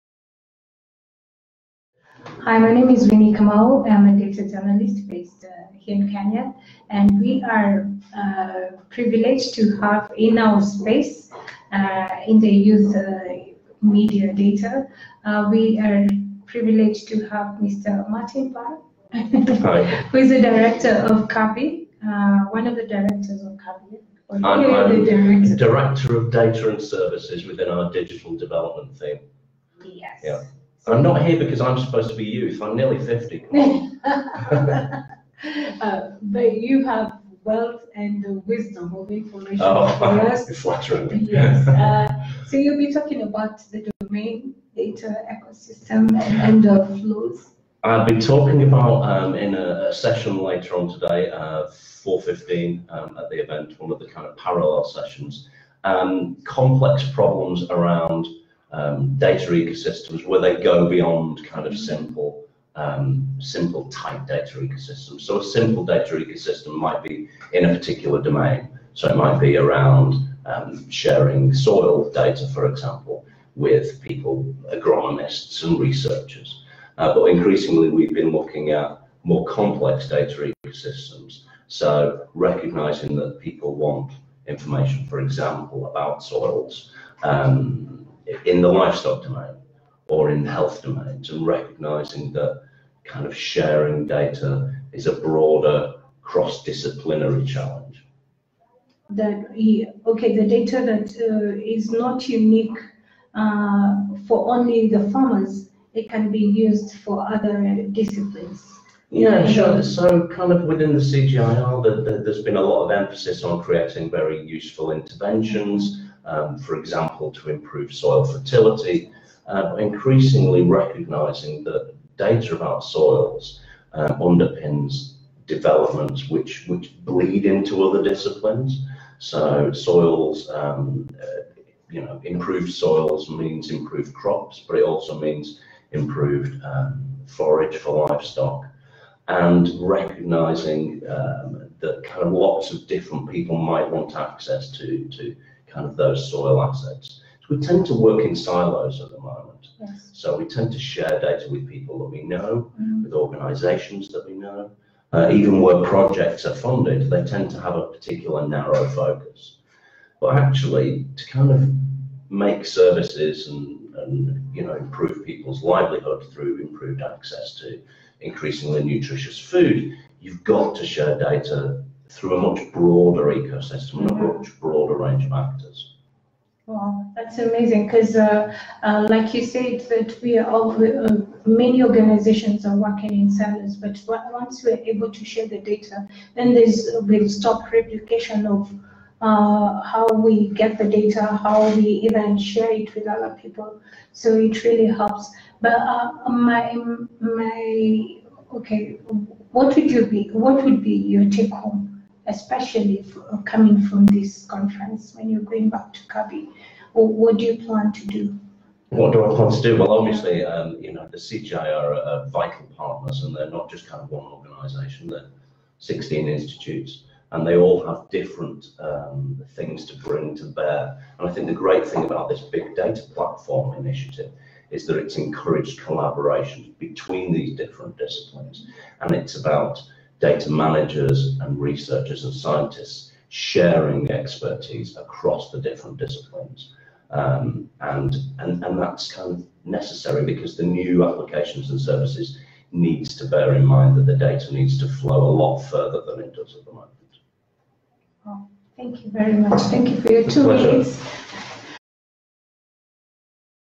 Q&A with Martin Parr from CABI - Decoding the Data Ecosystem
Q&A at the Big Data in Agriculture Convention: Decoding the Data Ecosystem - Nairobi, 3-5 Oct 2018. Martin Parr from Cabi chats with Winnie Kamau, one of our Youth in Data media delegates.